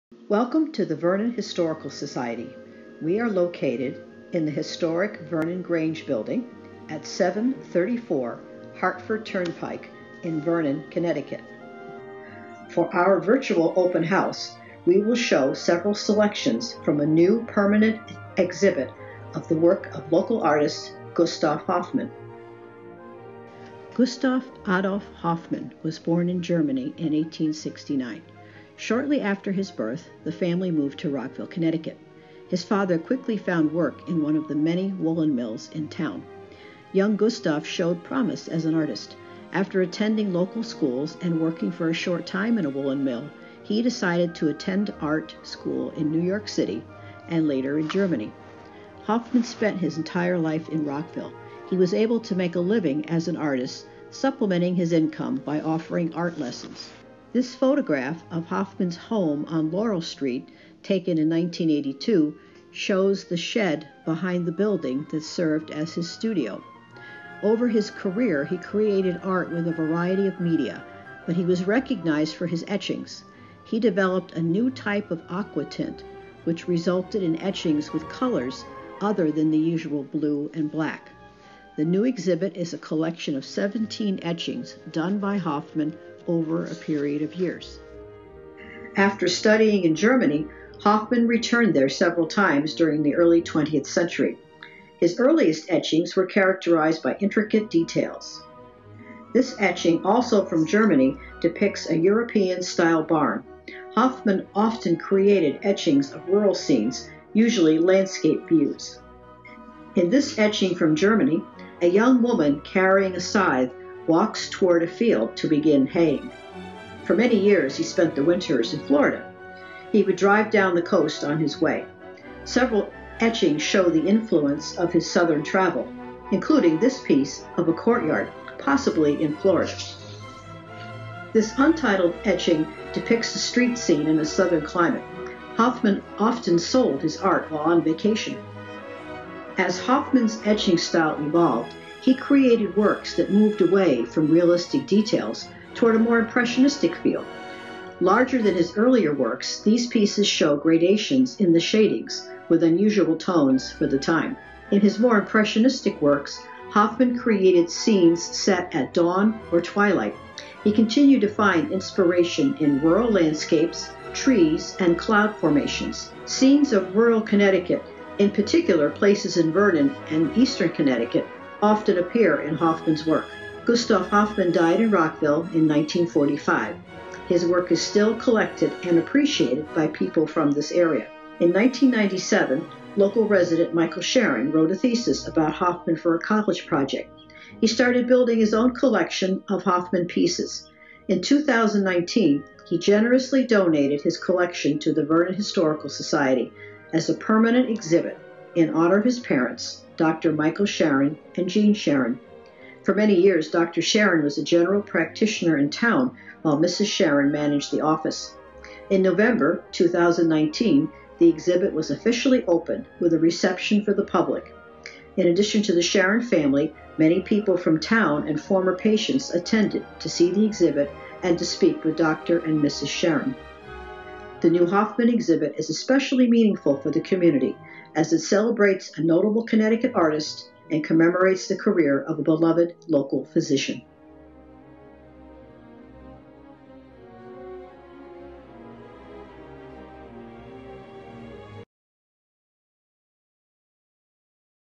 Gustave A. Hoffman Exhibit | Vernon Historical Society
The Vernon Historical Society welcomes you to our new rotating exhibition featuring local Rockville legend, Gustave Hoffman, and 17 of his etchings for Connecticut Open House Day 2020.